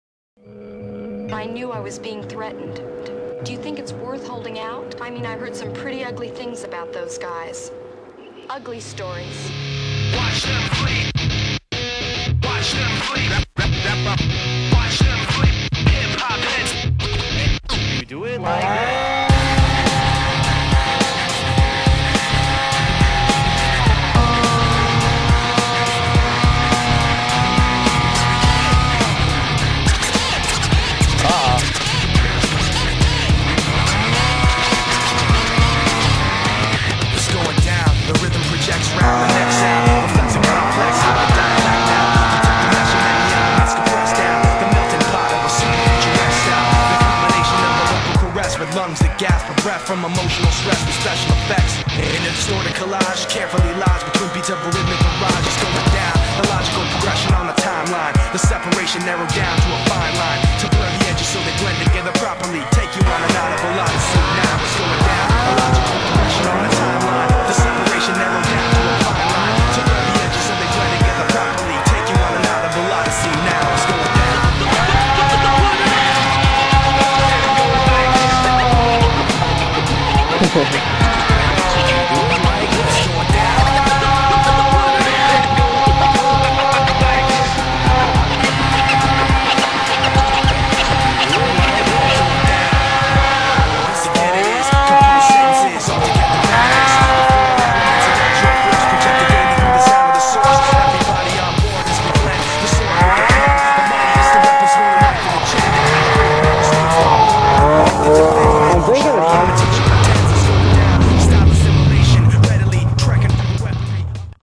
Maine short track riding -121"
Me and my friend riding in some new pow with our 121x1" tracks. Lay the hammer down!